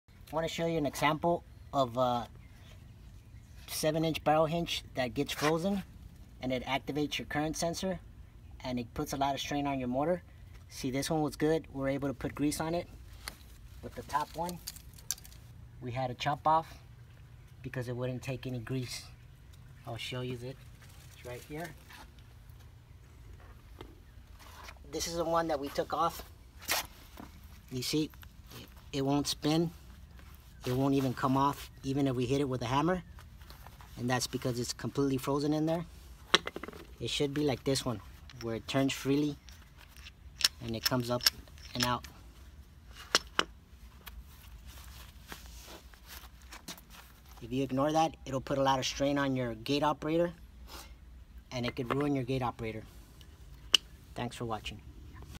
7 inch barrel hinges for swinging gates
Frozen (bad) 7 inch barrel hinge on swinging automated gate.